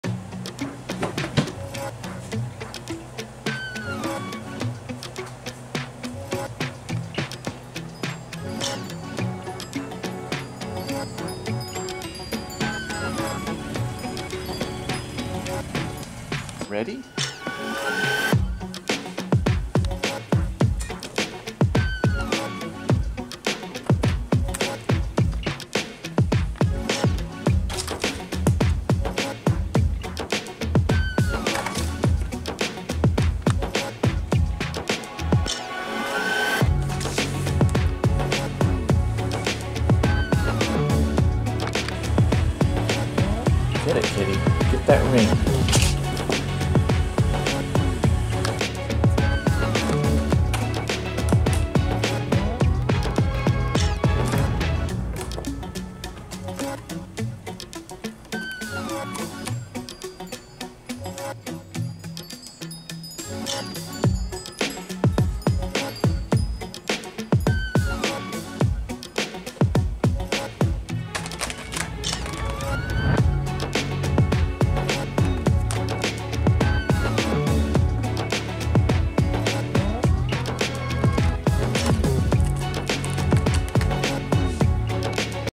She wants to jump higher and higher!
Here is some of what you missed while we were away. September 29 Maggie had fun with the ring and did progressively higher jumps! She actually prefers jumping up and sometimes over very high things outside like tree limbs, top of the privacy fence, window ledges, etc. It would be too nerve wracking for you to see that though and I don't want to encourage those extremes either. Lets just say that she is a little bottle rocket! So we do tamer ones inside instead to fulfill her urge to jump while being many degrees safer!

Love,
Maggie, Mike and Meowmie